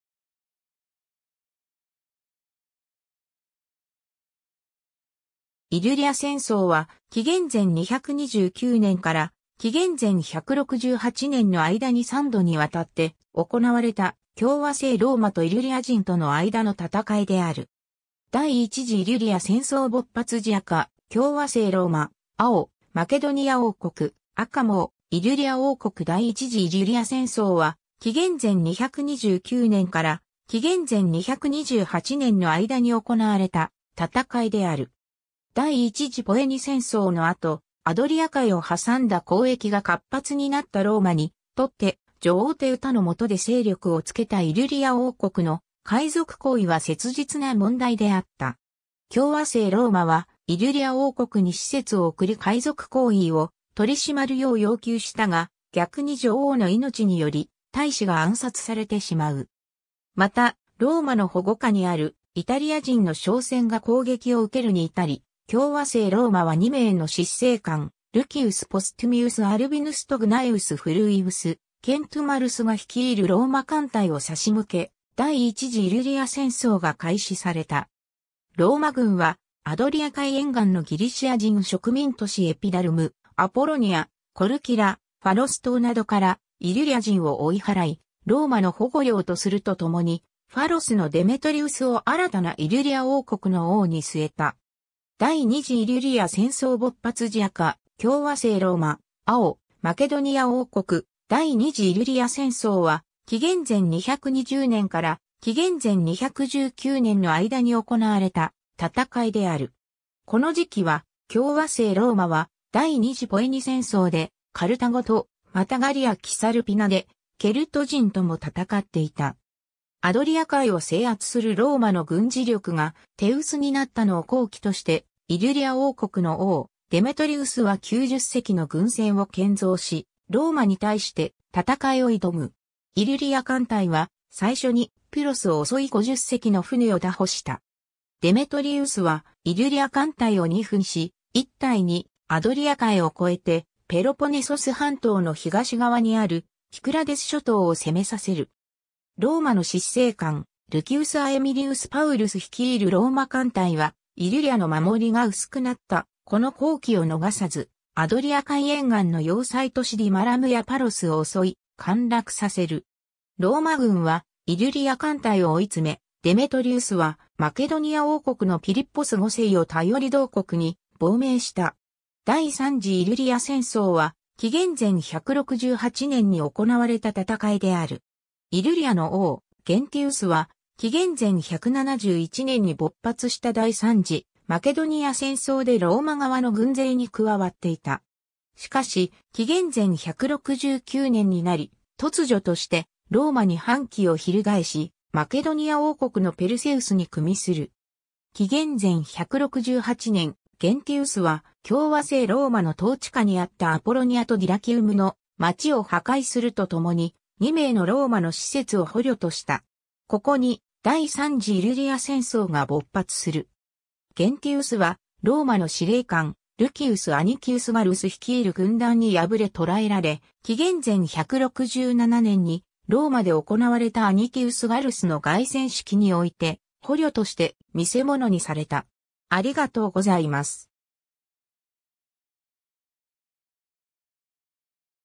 イリュリア戦争
イリュリア戦争（イリュリアせんそう、ラテン語: Bella Illyrica、英語: Illyrian Wars）は、紀元前229年から紀元前168年の間に3度に渡って行われた、共和政ローマとイリュリア人（アルディアエイ族）との間の戦いである。
 第一次イリュリア戦争勃発時 赤：共和政ローマ、青：マケドニア王国、赤網：イリュリア王国 第一次イリュリア戦争は紀元前229年から紀元前228年の間に行われた戦いである。
第一次ポエニ戦争の後、アドリア海を挟んだ交易が活発になったローマにとって、女王テウタの下で勢力を付けたイリュリア王国の海賊行為は切実な問題であった。
共和政ローマはイリュリア王国に使節を送り海賊行為を取り締まるよう要求したが、逆に女王の命により大使が暗殺されてしまう。
また、ローマの保護下にあるイタリア人の商船が攻撃を受けるに至り、共和政ローマは2名の執政官ルキウス・ポストゥミウス・アルビヌスとグナエウス・フルウィウス・ケントゥマルスが率いるローマ艦隊を差し向け、第一次イリュリア戦争が開始された。
ローマ軍はアドリア海沿岸のギリシア人植民都市エピダルム、アポロニア、コルキラ、ファロス島などからイリュリア人を追い払い、ローマの保護領とするとともにファロスのデメトリウスを新たなイリュリア王国の王に据えた。
 第二次イリュリア戦争勃発時 赤：共和政ローマ、青：マケドニア王国 第二次イリュリア戦争は紀元前220年から紀元前219年の間に行われた戦いである。
この時期は、共和政ローマは第二次ポエニ戦争でカルタゴと、またガリア・キサルピナでケルト人とも戦っていた。
アドリア海を制圧するローマの軍事力が手薄になったのを好機として、イリュリア王国の王デメトリウスは90隻の軍船を建造し、ローマに対して戦いを挑む。
イリュリア艦隊は、最初にピュロスを襲い50隻の船を拿捕した。
デメトリウスはイリュリア艦隊を2分し、1隊にアドリア海を越えてペロポネソス半島の東側にあるキクラデス諸島を攻めさせる。
ローマの執政官ルキウス・アエミリウス・パウルス率いるローマ艦隊は、イリュリアの守りが薄くなったこの好機を逃さず、アドリア海沿岸の要塞都市ディマラムやパロスを襲い陥落させる。
ローマ軍はイリュリア艦隊を追い詰め、デメトリウスはマケドニア王国のピリッポス5世を頼り同国に亡命した。
 第三次イリュリア戦争は紀元前168年に行われた戦いである。
イリュリアの王ゲンティウスは、紀元前171年に勃発した第三次マケドニア戦争でローマ側の軍勢に加わっていた。
しかし紀元前169年になり、突如としてローマに反旗を翻しマケドニア王国のペルセウスに与する。
紀元前168年、ゲンティウスは共和政ローマの統治下にあったアポロニアとディラキウムの街を破壊するとともに、2名のローマの使節を捕虜とした。
ここに第三次イリュリア戦争が勃発する。
ゲンティウスはローマの司令官（プロプラエトル）ルキウス・アニキウス・ガッルス率いる軍団に敗れ捉えられ、紀元前167年にローマで行われたアニキウス・ガッルスの凱旋式において捕虜として見世物にされた。